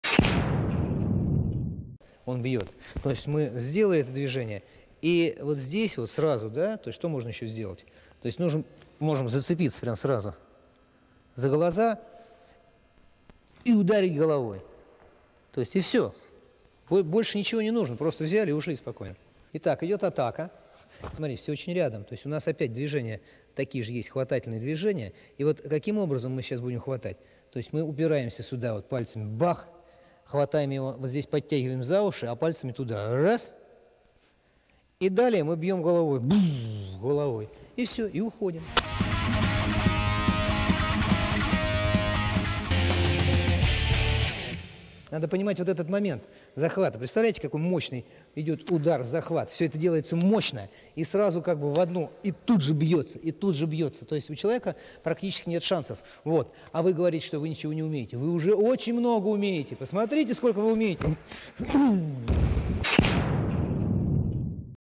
Удар головой в самообороне.Вариант.В Н Крючков/selfdefence. header. V.Kryuchkov
Один из множества вариантов удара головой в ближнем бою. В мою молодость очень распространённый был удар. И всегда имел 100% успех. Вариантов, как я сказал ,  множество и как использовать удар , каждый решает сам. Здесь всего одно основное условие - ближняя дистанция!